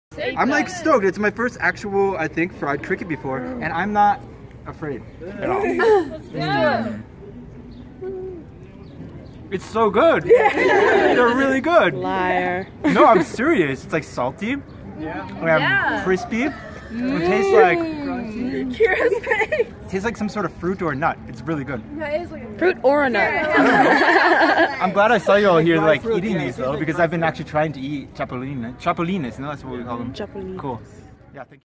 Chapulinas - eating fried crickets, and they're sooo good.
Filmed in the city of Chalula, Puebla, Mexico